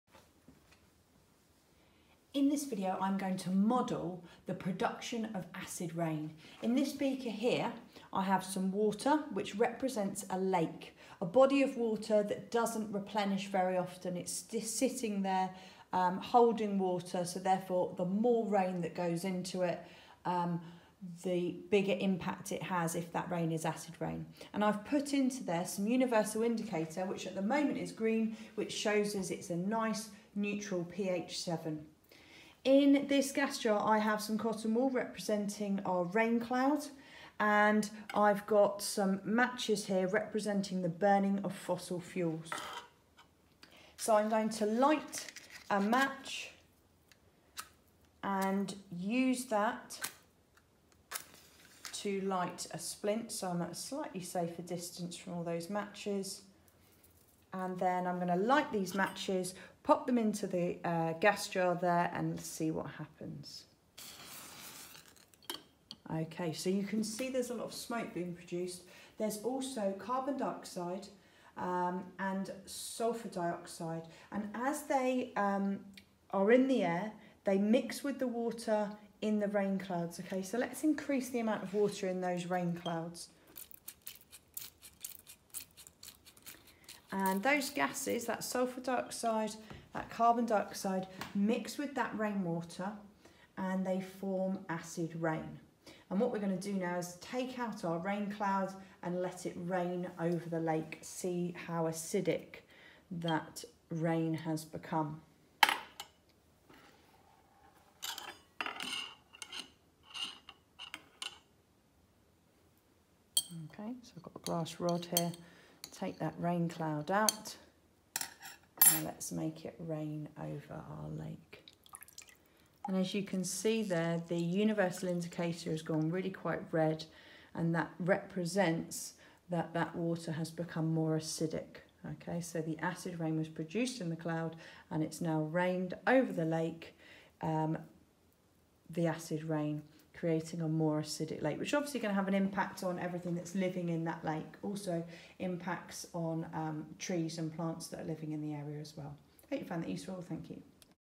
Chemical Weathering - Acid Rain
Key stage 3 Science and Year 4 rock cycle, looking at the production of acid rain.